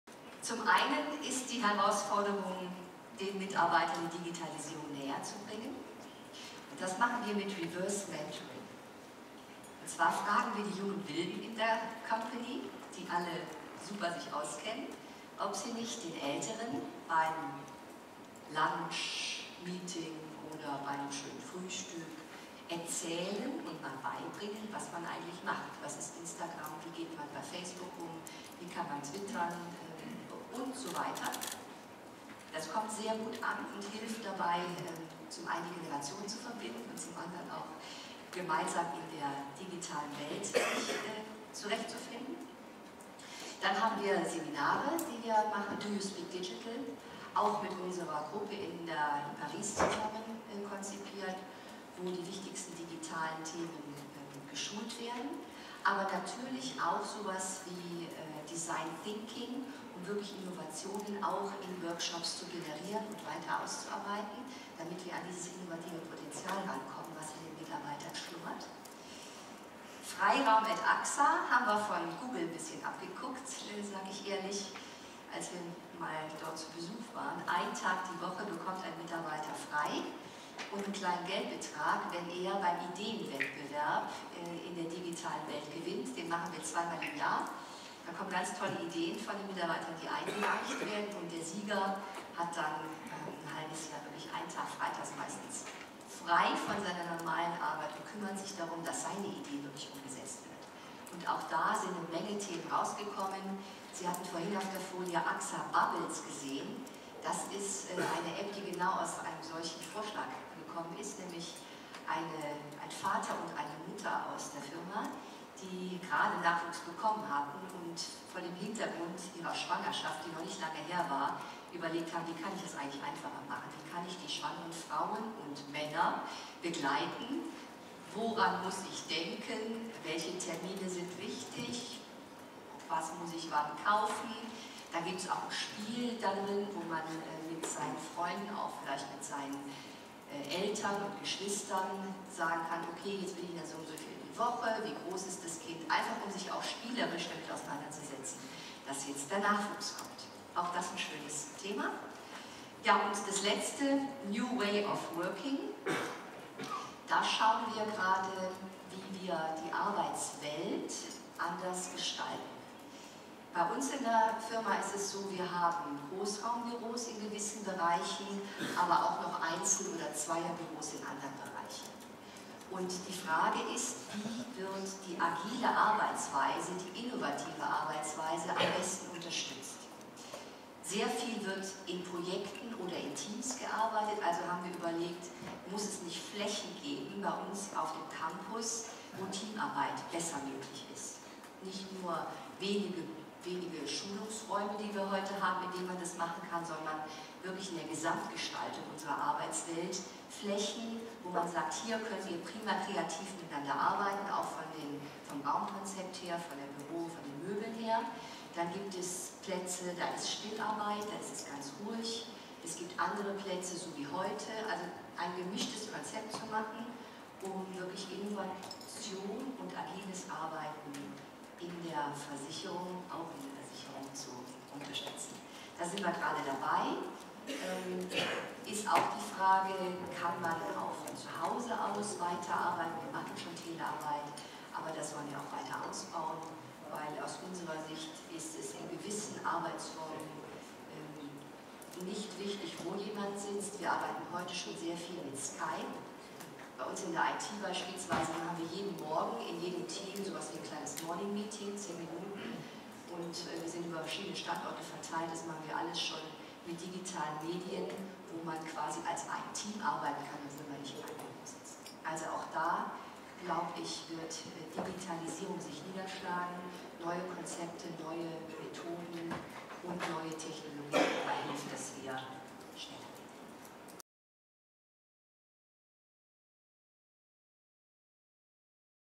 Axa fragt die Mitarbeiter: "Do you speak digital?"
"Digitalisierung" ist in aller Munde, auch bei dem französischen Versicherungsunternehmen Axa. In ihrem Vortrag auf der diesjährigen Inscom in München, alle zwei Jahre vom IT-Beratungshaus MSG veranstaltet, gab Axa-Vorstand van Aubel einen Einblick in ganz konkrte Maßnahmen, die das Unternehmen im Rahmen der digitalen Revolution angeschoben hat. Die Aktivitäten kumulieren in der Frage an die eigenen Mitarbeiter: "Do you speak digital?"